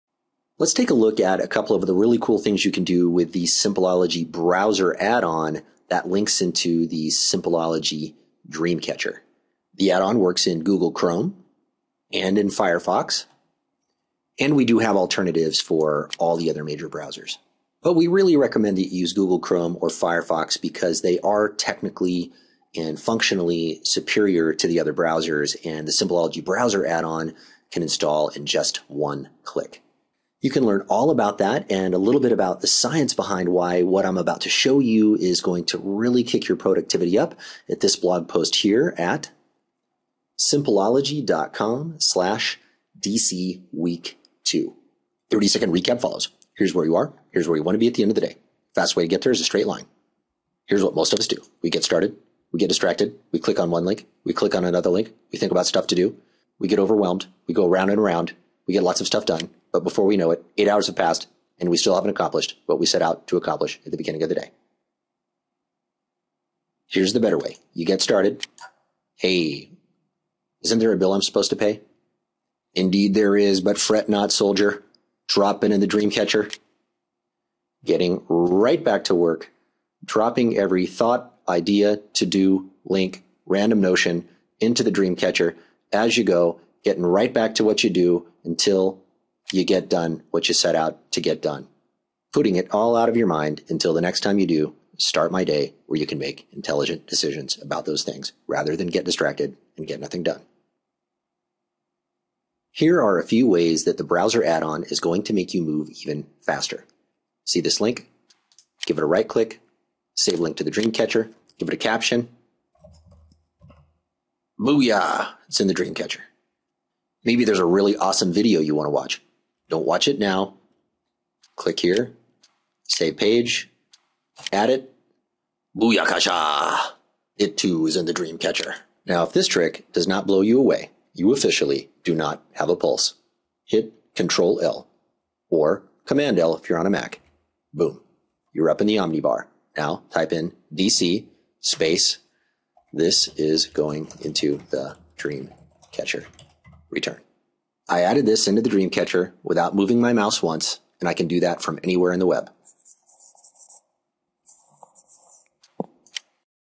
How to Use the Simpleology Browser Add-On to Kick Procrastination in the (CENSORED)
Here are some things you probably didn't know you could do with the Simpleology Browser Add-On. Kick procrastination and distraction in the (shut yo' mouth!).